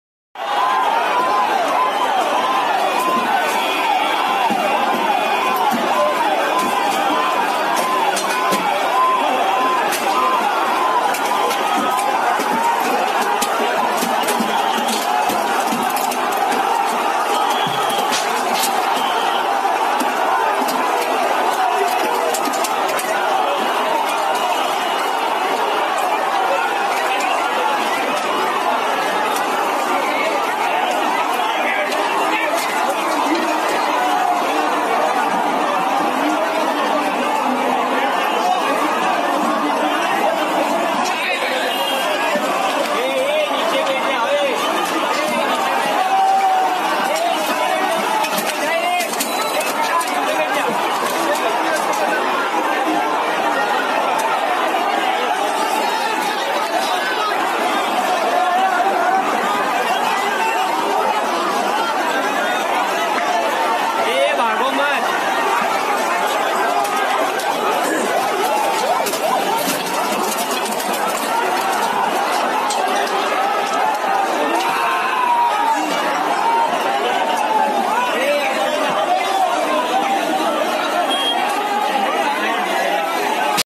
👆🏻மக்கள் தெருவில் இறங்கி விட்டனர். நரேந்திர மோடியின் போலீசும் இராணுவமும் தாக்குப்பிடிக்க முடியாமல்
👆🏻மக்கள் தெருவில் இறங்கி விட்டனர். நரேந்திர மோடியின் போலீசும் இராணுவமும் தாக்குப்பிடிக்க முடியாமல் ஓடி ஒளியும் காட்சி